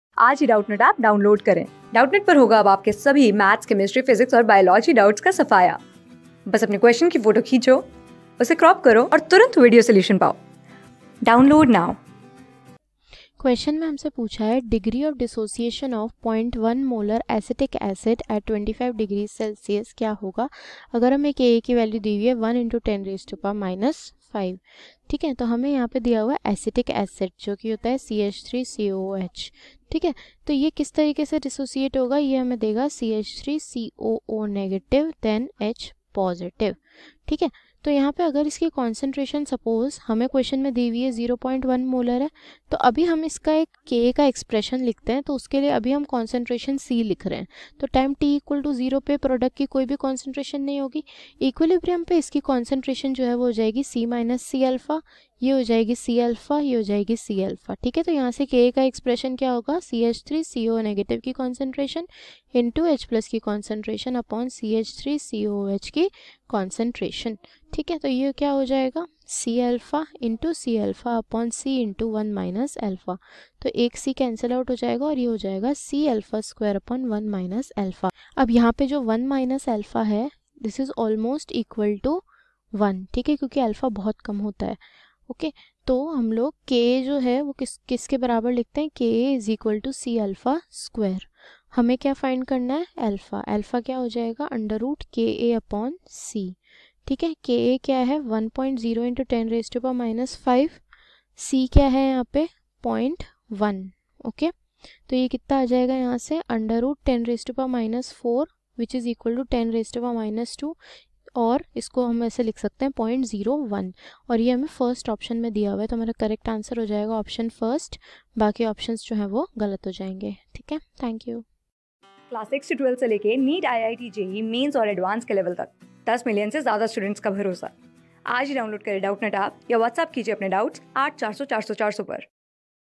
Degree of dissociation of 0.1 molar acetic acid at 25^(@)C (K_(a) = 1.0 xx 10^(-5)) is | CLASS ...
Degree of dissociation of 0.1 molar acetic acid at 25^(@)C (K_(a) = 1.0 xx 10^(-5)) is
Class: 12
Subject: CHEMISTRY
Chapter: ICONIC EQUILIBRIUM
Board:IIT JEE

👉 Have Any Query? Ask Us.